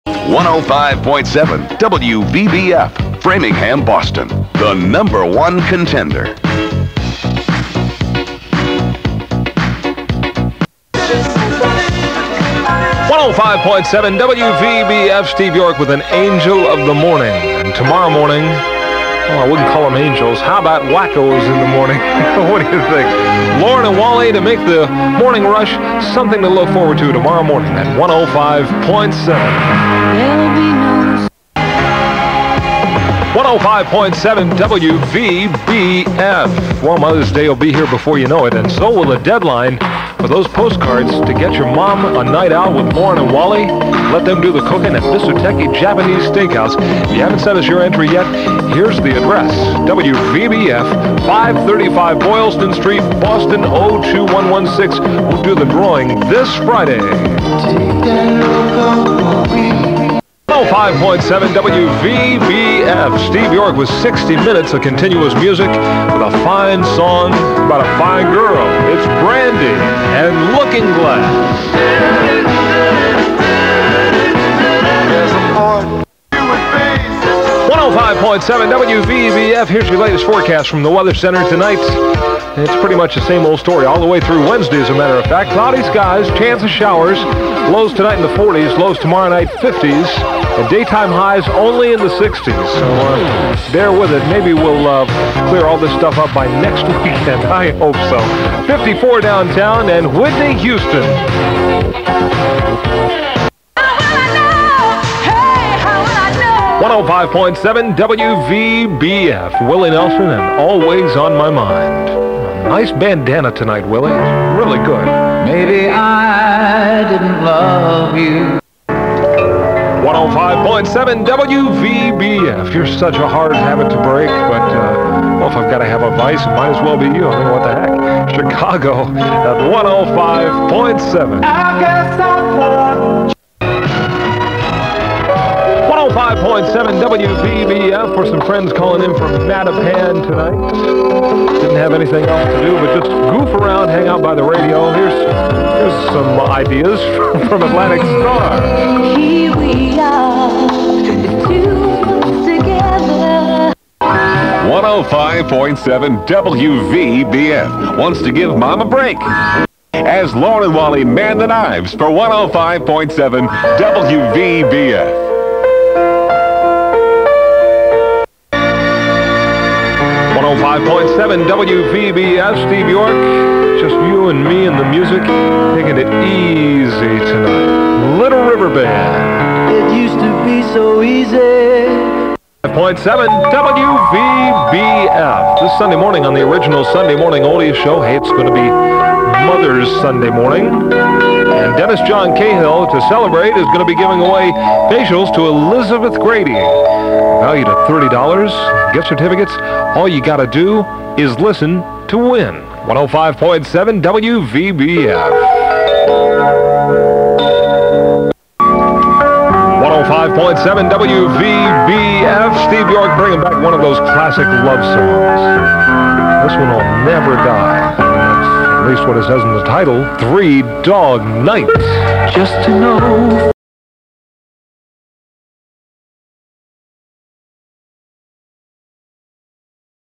105.7 WVBF Boston - May 1986 aircheck
Scoped aircheck featuring Steve York.  WVBF was one of those rare AC stations that had great personality too.